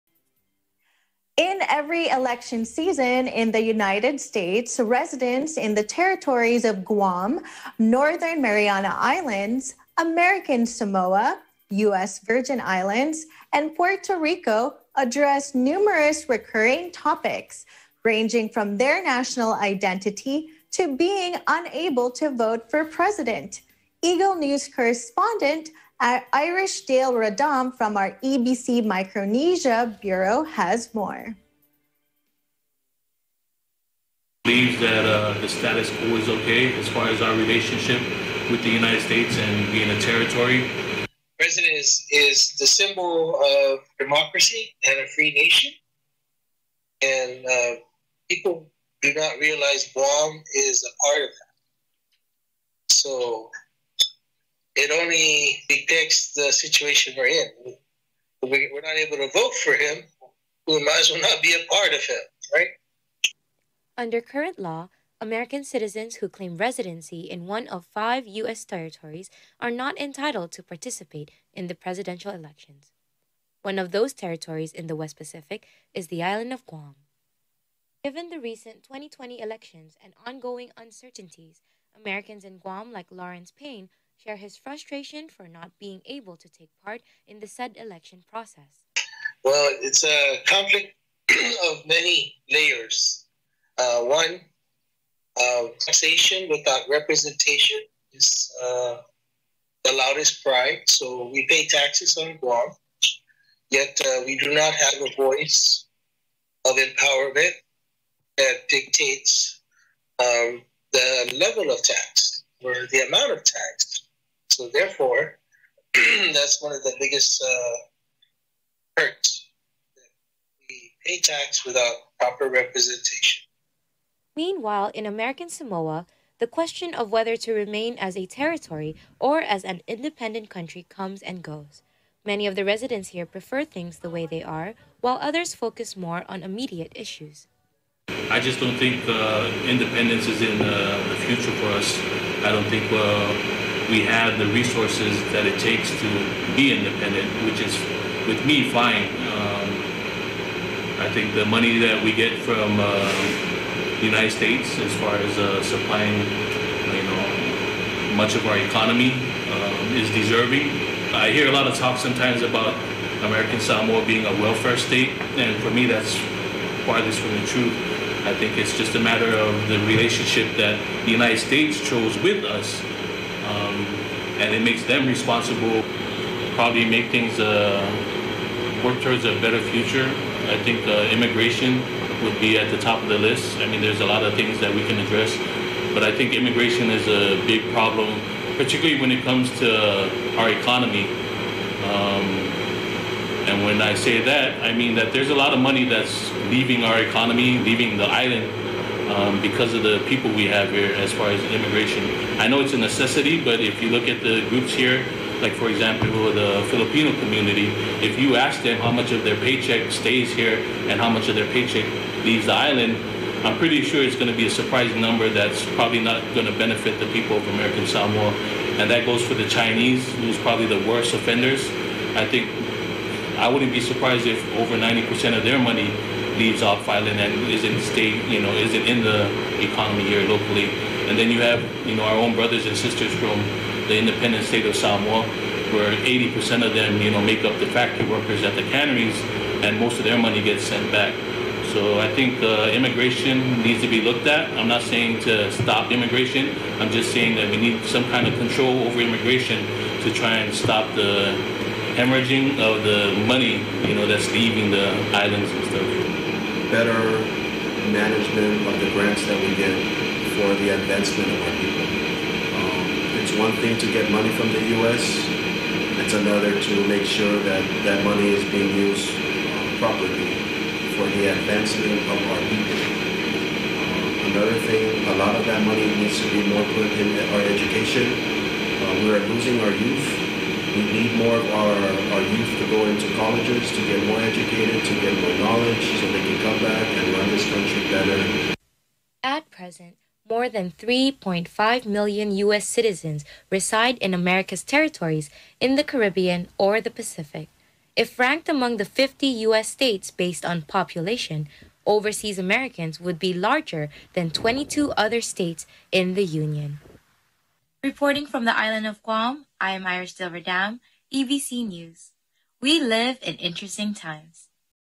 Americans in overseas territories share thoughts on recurring issues during elections
In every election season in the United States, residents in the territories of Guam, Northern Mariana Islands, American Samoa, US Virgin Islands, and Puerto Rico address numerous recurring topics, ranging from their national identity to being unable to vote for president. Eagle News Correspondent Irishdale Radam from our EBC Micronesia Bureau reports.